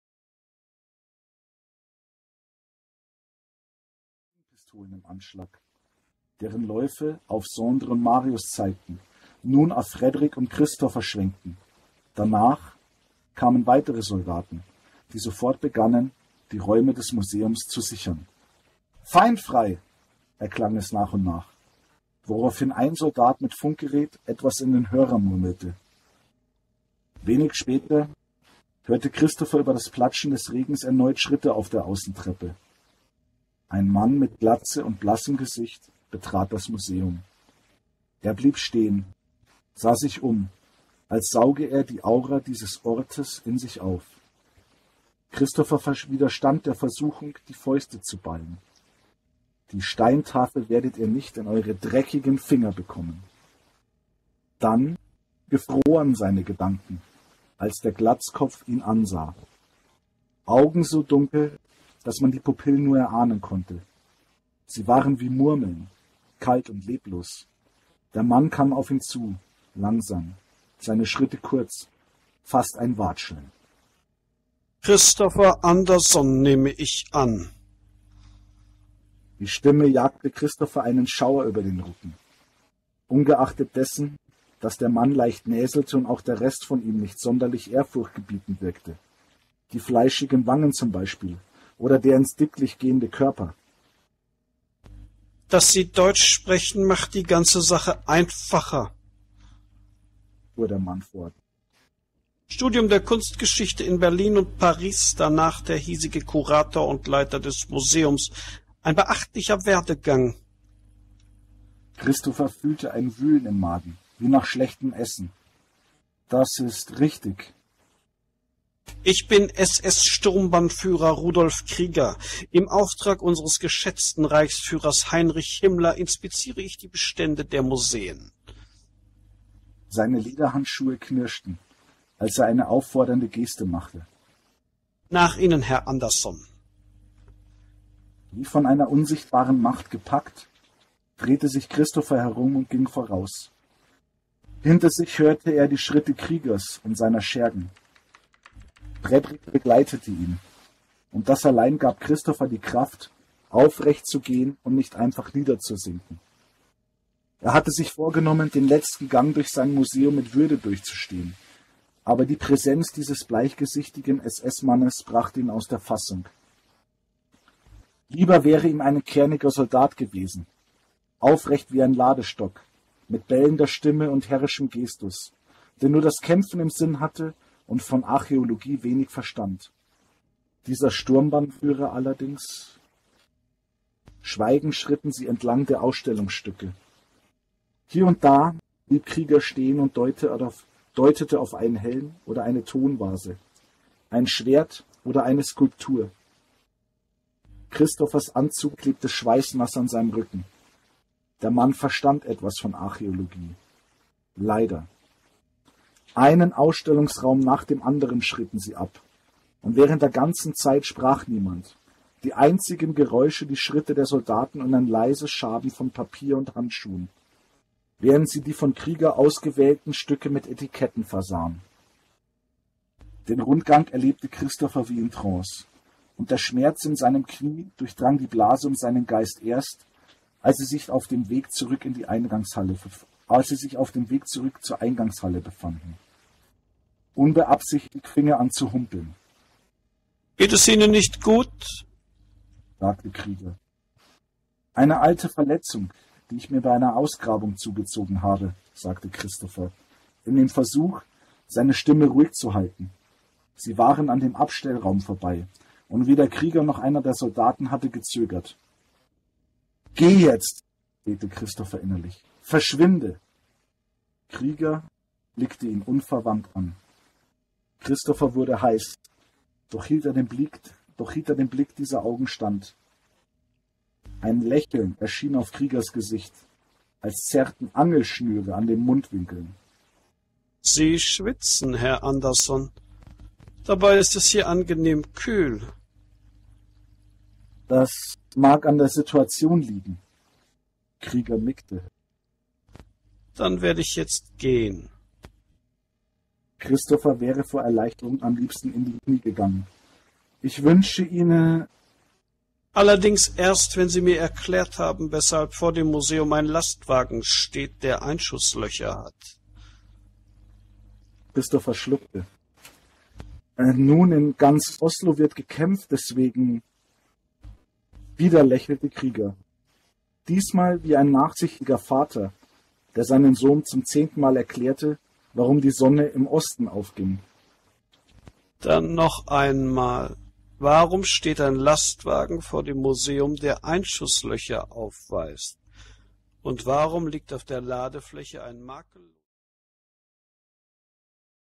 Peter Hohmann liest live aus "Operation Thule"
Peter Hohmann bei seiner Lesung aus "operation Thule".
Bühnenbild: Barlok Barbosa
Audio- und Videomitschnitt, sowie Editing: Clairediluna Chevalier
Introbild: BukTom Bloch